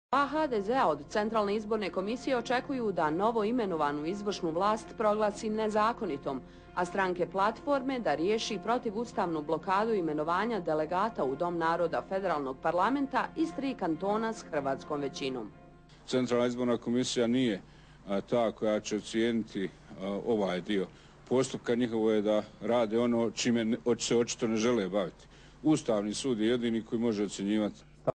SDP BiH BHT Crta Nermin Niksic
Nermin Niksic govori o potezima HDZ-a i CIK-a